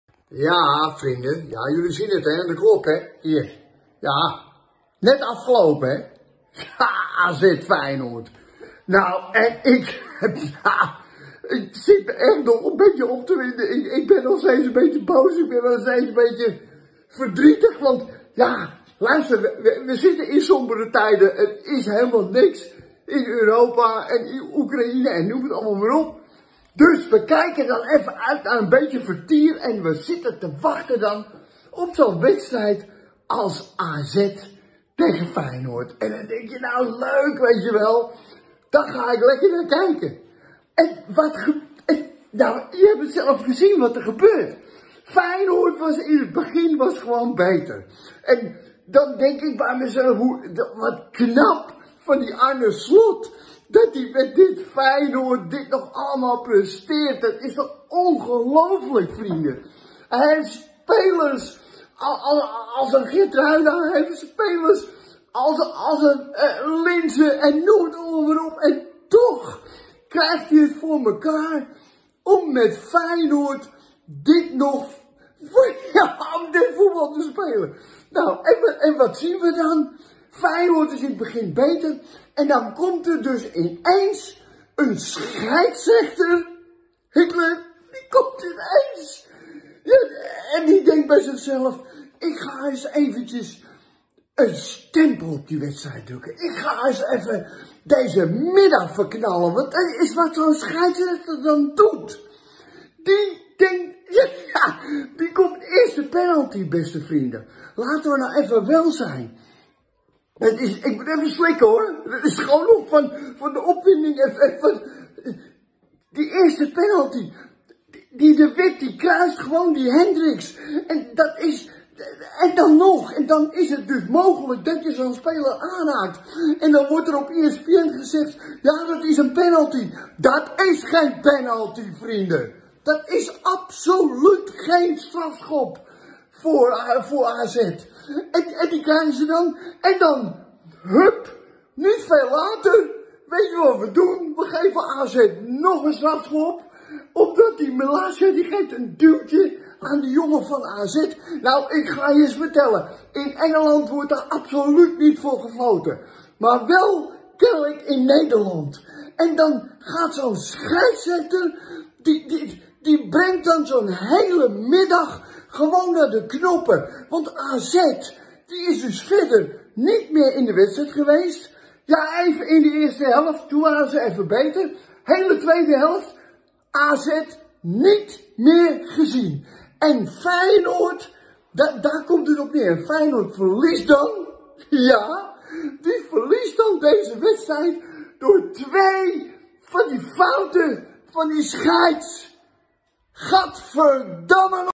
Scheids beslist AZ-Feyenoord!
Misselijk! Die scheids bij AZ-Feyenoord! Beslist de wedstrijd met z’n penalty’s!!!